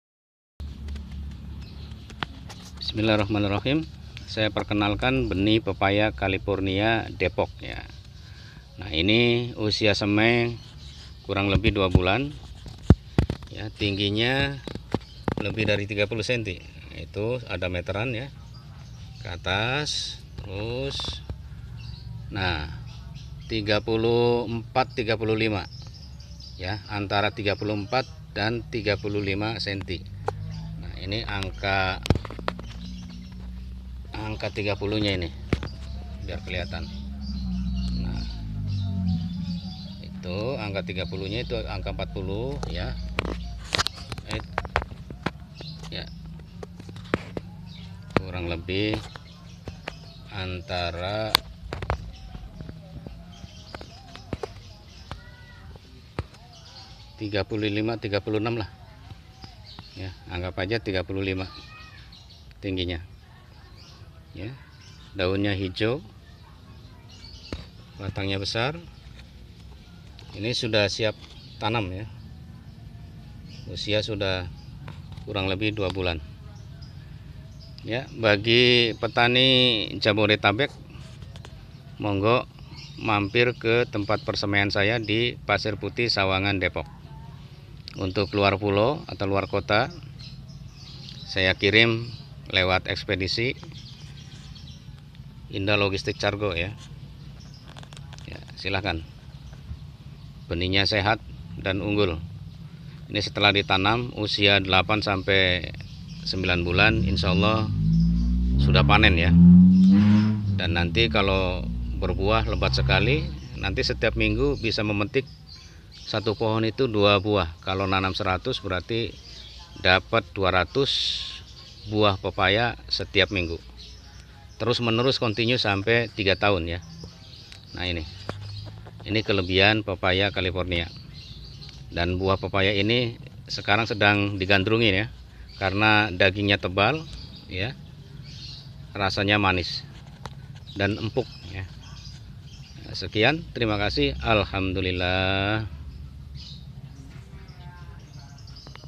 Episode 14. Bibit Pepaya California Depok. WA: 081293397078
Bibit Pepaya California Depok, siap melayani permintaan bibit pepaya California ke seluruh Indonesia.

Usia bibit 1-2 bulan, tinggi bibit 15-40 cm, siap ditanam dilahan kebun atau di pot besar.

Harga mulai dari Rp.5000-15.000/pohon, diluar ongkir.

Bagi yg domisili di wilayah Jabodetabek bisa langsung merapat ke tempat persemaian saya di:

Lokasi Persemaian:
Pasirputih Sawangan Kota Depok Jabar.

Yang berminat silahkan hubungi kami:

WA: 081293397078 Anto